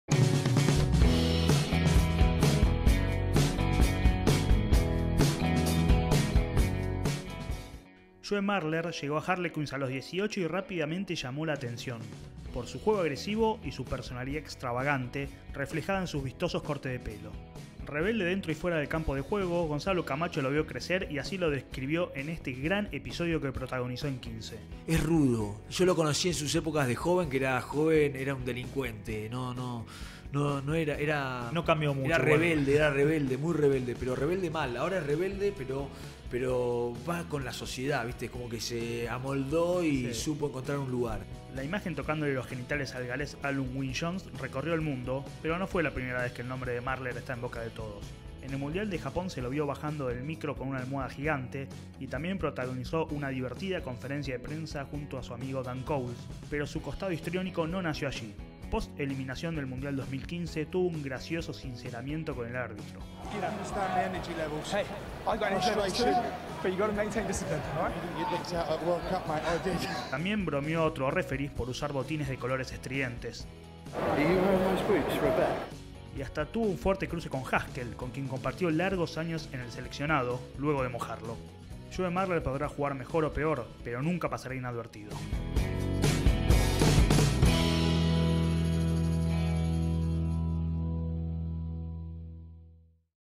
XV PODCAST TV - JOE MARLER
Conocé un poco más de la historia del divertido, polémico y extravagante pilar inglés Joe Marler.